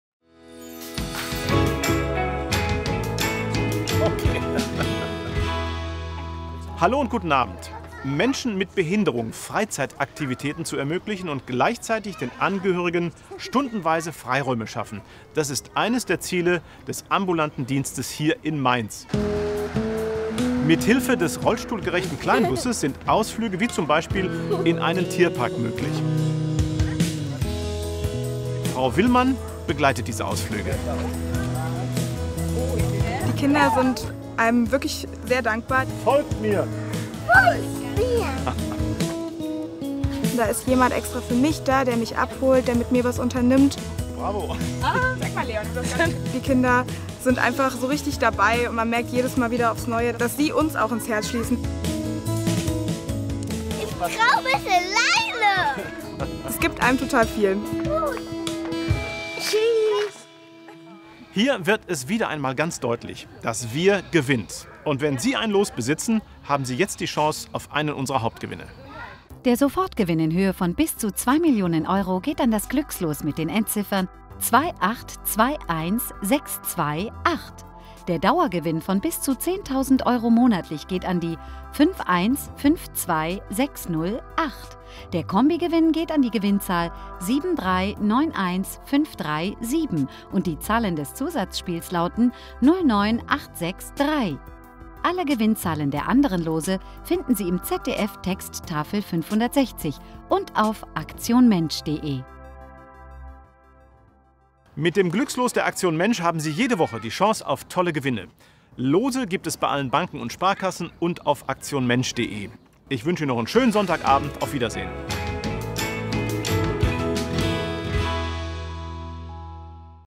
Aktion Mensch-Gewinner vom 02. August 2015: Kleinbus Mainz
Rudi Cerne präsentiert die Gewinnzahlen der Aktion Mensch und stellt das Projekt "Ambulanten Dienste der Körper- und Mehrfachbehinderte Mainz gGmbH" vor.

Wo soll's heute hingehen? Ausflüge in den Streichelzoo, zum Bowling oder ins Aquarium sind in Mainz durch die "Ambulanten Dienste der Körper- und Mehrfachbehinderte Mainz gGmbH" auch für Kinder und Jugendliche mit Behinderung möglich. In dem rollstuhlgerecht umgebautem Kleinbus können sie barrierefrei ihre Umgebung erkunden und mobil sein.

Die Aktion Mensch unterstützt die Anschaffung eines rollstuhlgerechten Kleinbusses mit 54.000 Euro.